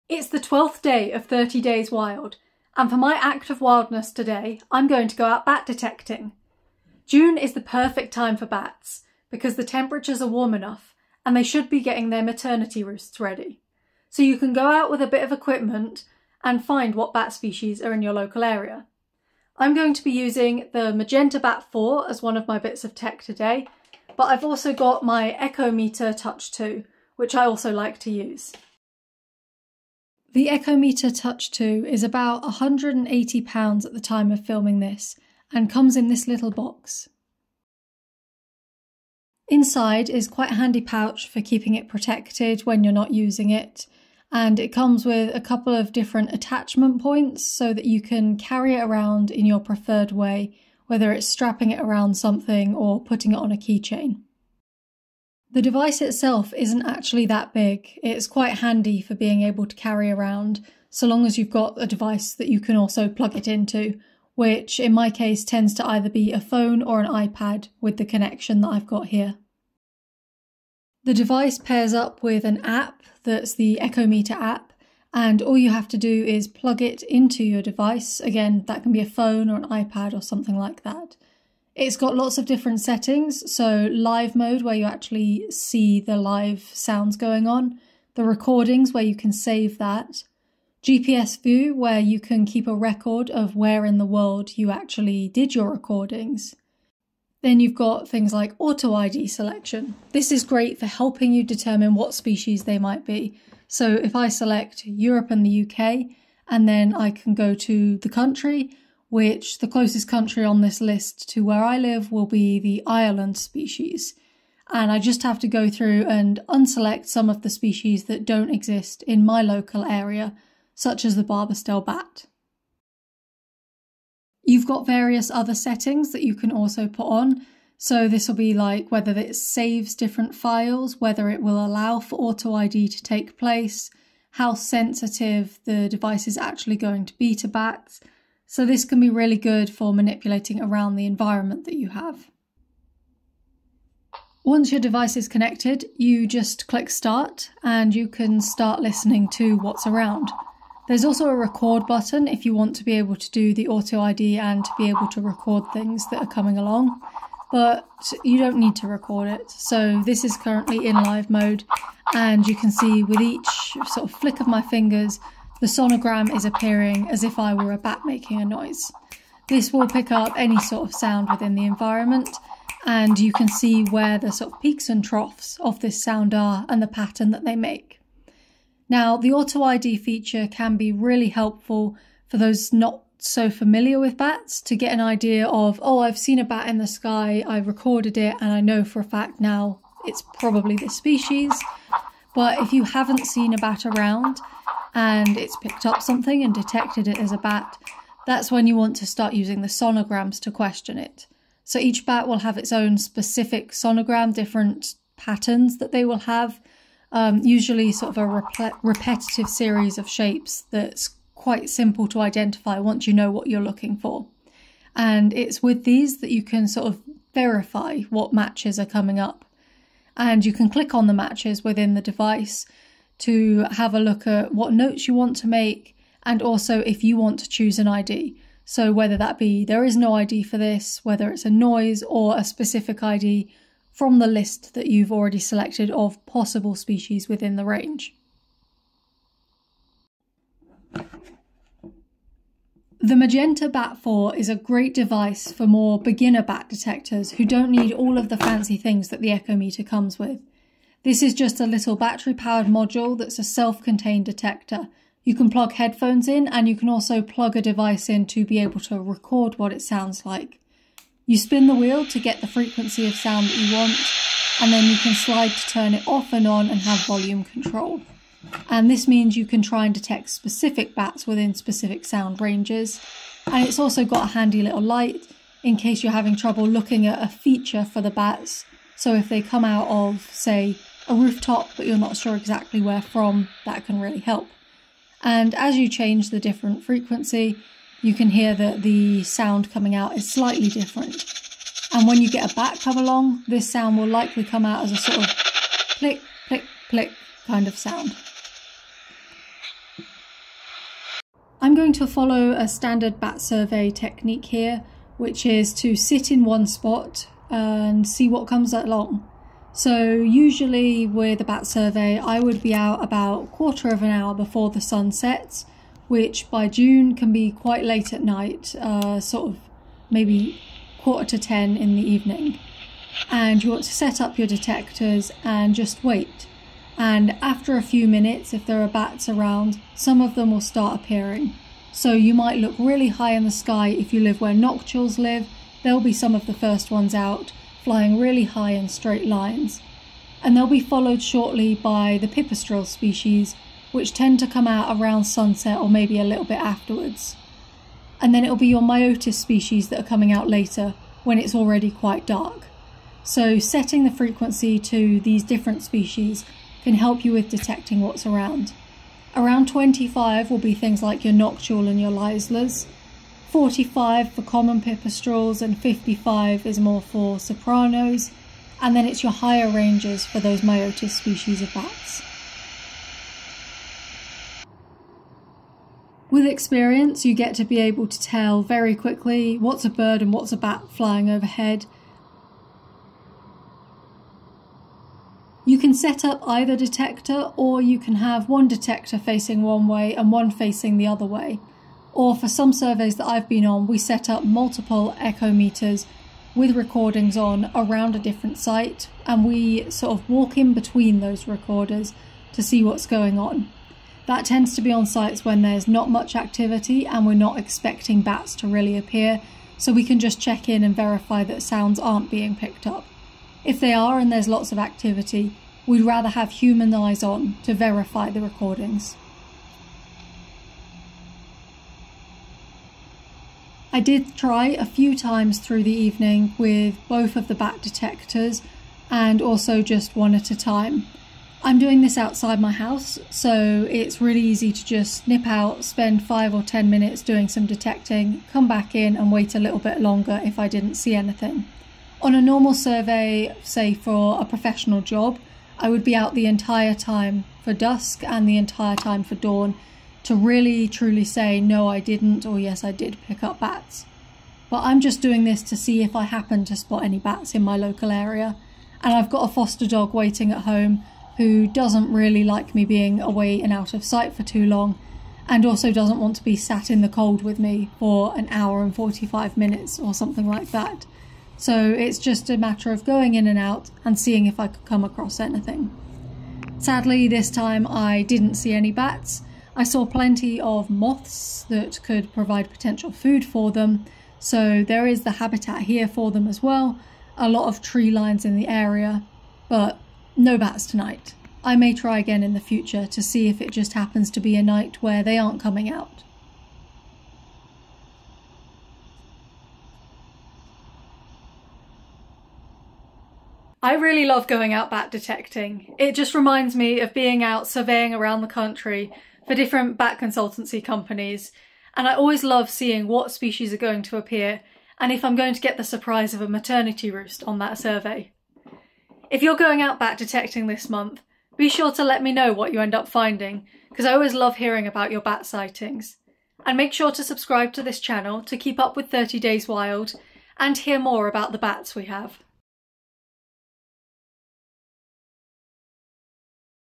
Bat detecting with the echo meter touch 2 and magenta bat 4 during 30 Days Wild
On day 12 of 30 Days Wild I'm revisiting one of my first fieldwork skills: bat detecting! I love going out looking for bats but haven't had the chance to do much of it recently. So, I'm taking to my garden to see if there are any bats living around my neighbourhood.